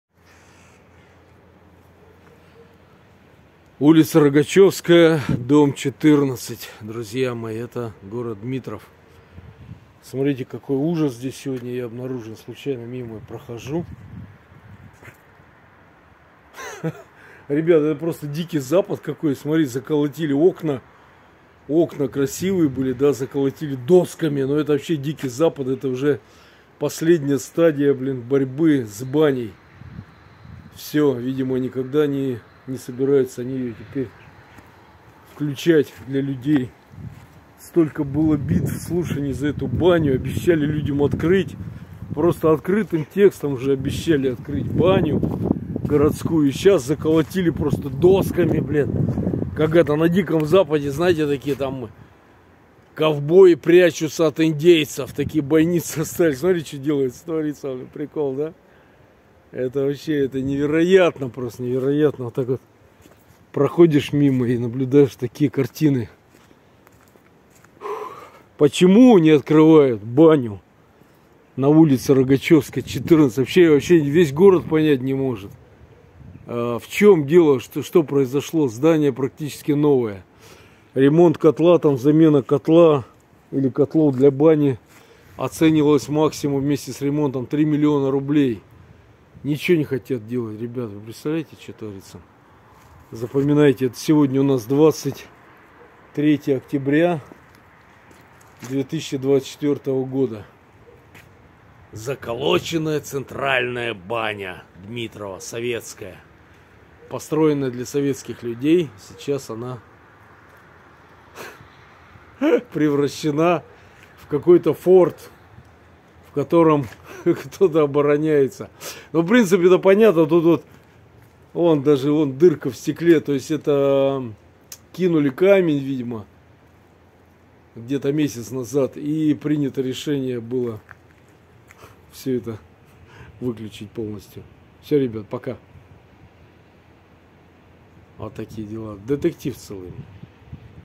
Городская баня в Подмосковном Дмитрове, это дикий запад какой то
Страшная трансформация бывшей городской бани в Дмитрове. Теперь это словно Дикий вестерн какой-то.
      Почти фантастическая история писателя-фантазера и режиссера Александра Елизарэ с поэтом и прозаиком Вальдемаром Архиповым журналистом Московской Правды.
      А этот фильм о приключениях на съемочной площадке фильма Дядя Витя и ВиктОр Бон-Бон.
      ВиктОр, отъявленный хулиган и задира,  он представляет себя бригадным комиссаром полиции Парижа,  который  ищет фантастического Африканского Синего льва. Комиссара  преследуют  разного рода жулики, бандиты и авантюристы. Они вылезают из его романов и начинают  свою игру. ВиктОру помогают его друзья: мадемуазель Маруся и пьяница Антуан Матис.